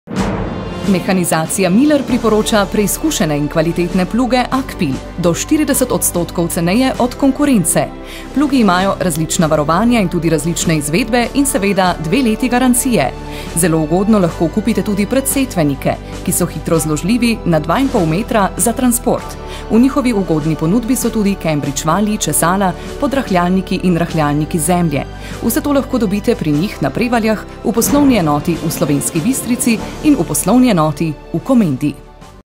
Reklama Mehanizacija Miler plugi.wmv
Mehanizacija Miler d.o.o. predstavlja stroje za obdelavo tal proizvajalca AKPIL.